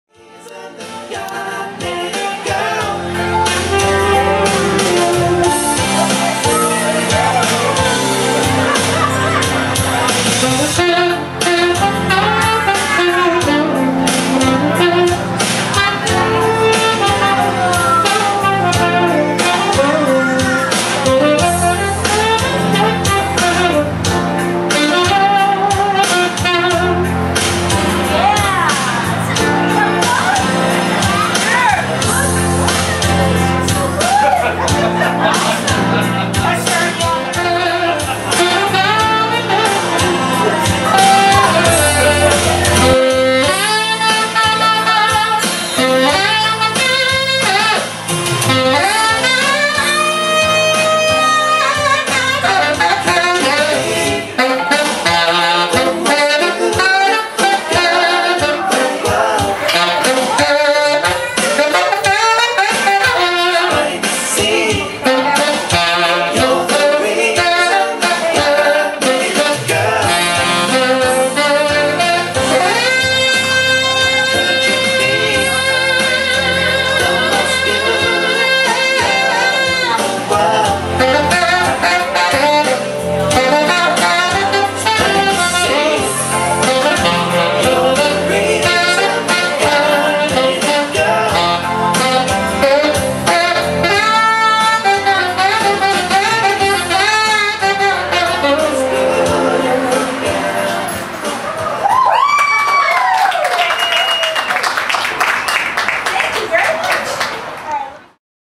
Ava Interacts with the Audience
Ava Lemert the Singing Saxstress™ Live from Ettore's in Sacramento
visit Ava for dates, music & to hire her for your event and/or venue